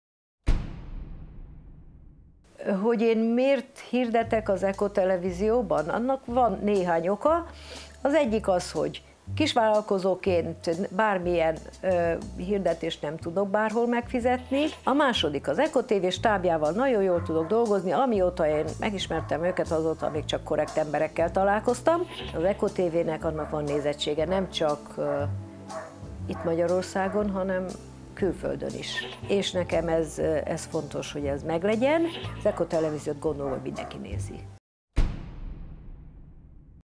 Miért érdemes hirdetni az Echo TV-ben?
Miért érdemes hirdetni az Echo TV-ben? Partnereink elmondják!

Tafedim Tea - Tamag Bt.